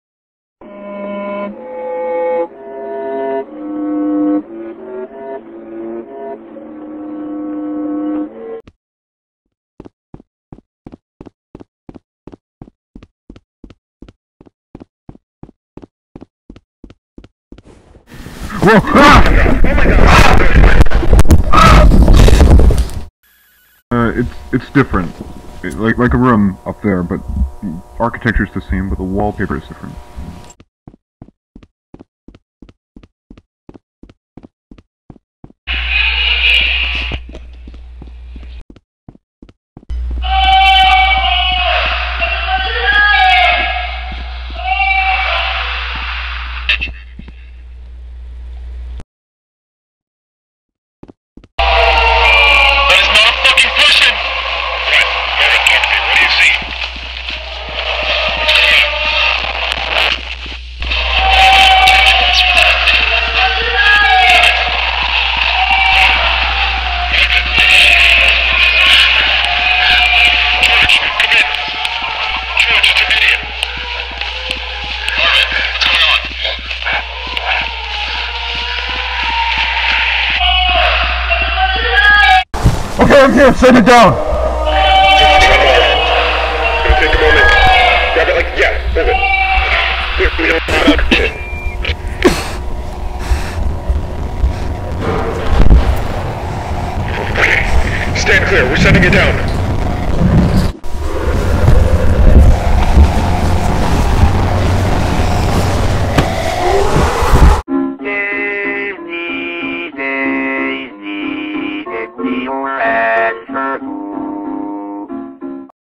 Backrooms - Pitfalls [Entity Encounter]
This vhs tape was recorded by Hazmat █████. Any civilians viewing things outside the A-Sync Facility will be punished or terminated